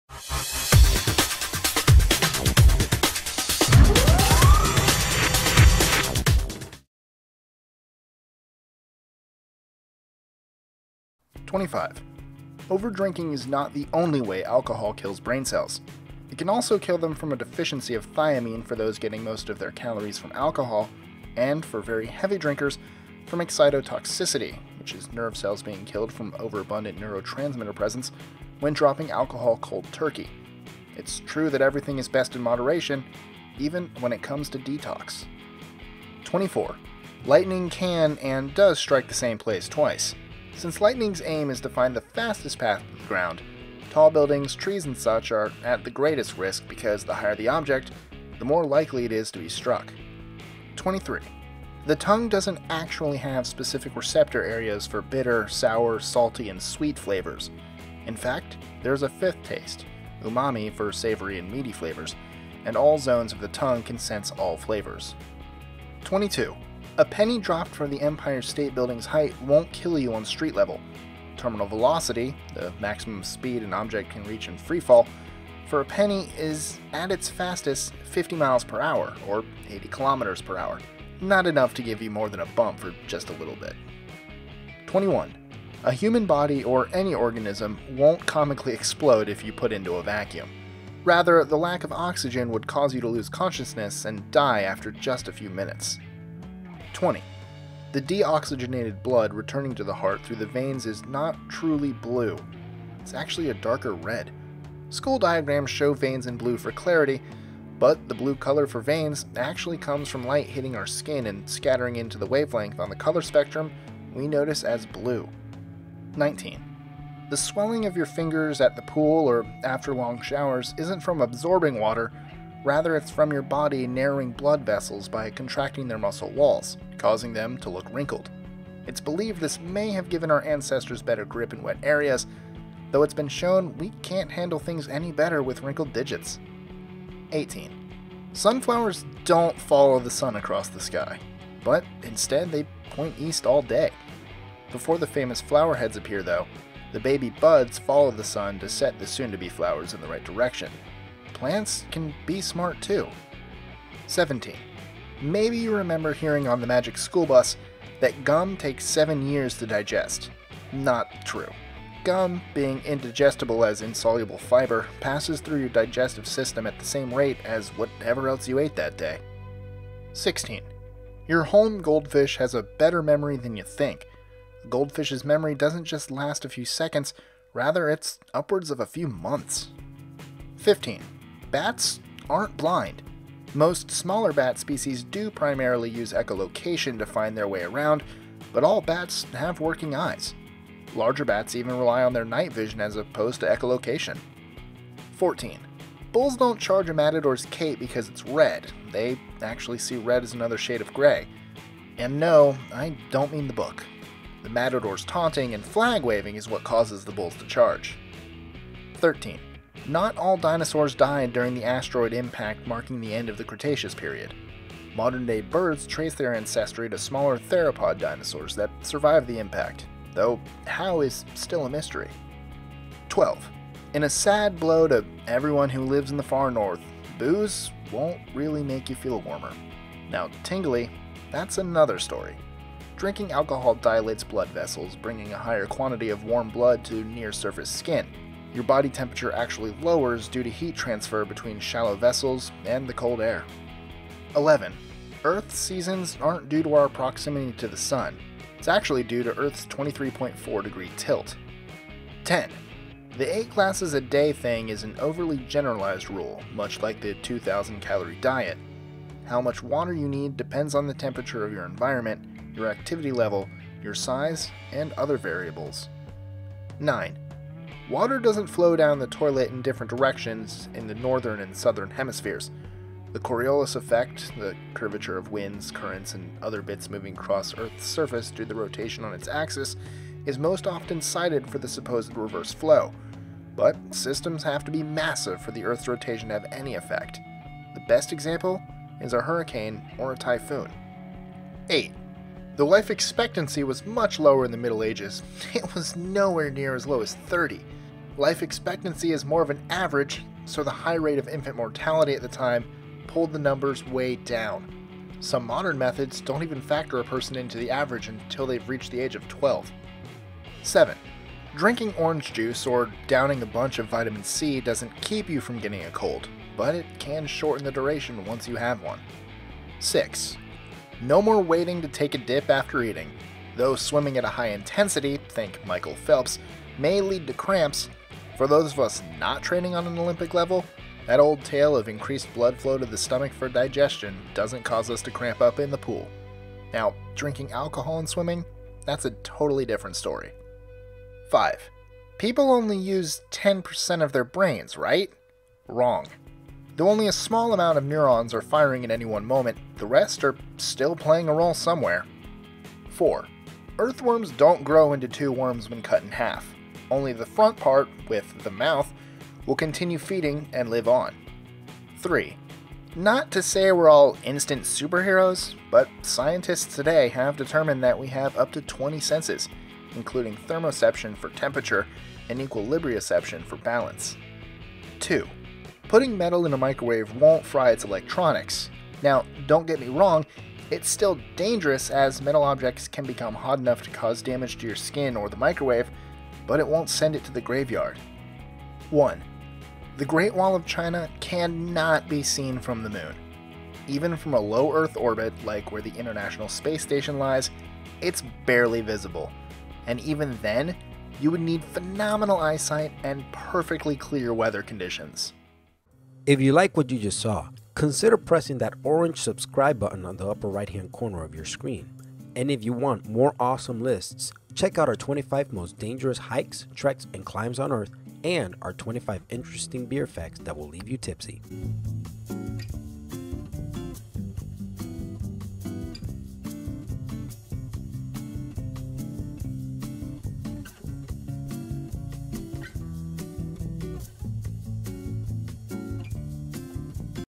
25 Science “Facts” That Were Proven Wrong
We’re taught loads of scientific facts formally or informally from an early age. And they’re all said to be true and reinforced by our common sharing of them. As time progresses, scientists use new methods to find some of those facts (or, in some cases, rumors that are so pervasive they enter the common psyche as perceived true facts) wrong. In this list, we break down some so-called “facts” we believed to be true and show what the science really says.

Preview:

Heavy drinking kills brain cells
Lightning doesn't strike the same place twice
The tongue has specific taste zones
A penny dropped from the Empire State Building can kill you
Being in a vacuum causes our eyes to pop out
Blood in your veins is blue
Your fingers swell up from water
Sunflowers follow the sun across the sky
Gum takes 7 years to digest
Goldie the fish has a memory of 5 seconds
Bats can't see
Bulls are driven mad by red
The asteroid impact killed off all the dinosaurs
Drinking alcohol warms you up
Seasons are due to how close we are to the sun
Drink 8 glasses of water a day
Water flows the opposite way in the different hemispheres